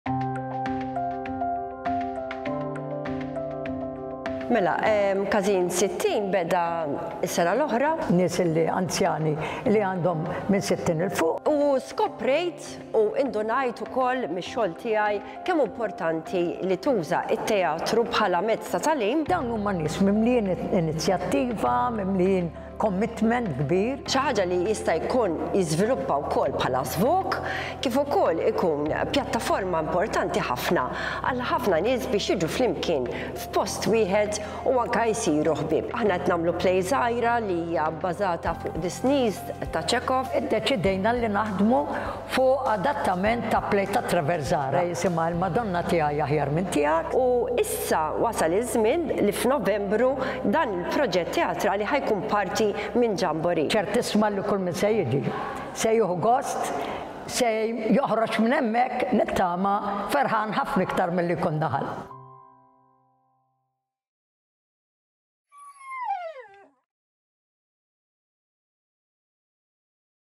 🇲🇹 ĦRF/XTW 2023-2024 | Il-Festival għall-anzjani li ma kibru qatt!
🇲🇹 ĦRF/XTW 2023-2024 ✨

Il-Festival għall-anzjani li ma kibru qatt! Josette Ciappara u Charlotte Stafrace jitkellmu dwar l-ispettakli li ħa jtellaw waqt Ġambori. Dan il-festival ħa jkun prototip li għandu d-destin li jkun avveniment annwali għalina.

Nisperaw li narawkom!✨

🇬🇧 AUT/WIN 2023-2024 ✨

A festival for 60+ individuals who are still young at heart! Josette Ciappara and Charlotte Stafrace will be whocasing two performances during Ġambori. A prototype of an active-aging performing arts festival, launching at St Vincent De Paul Theatre, with plans for further development.

Hope we see you there! ✨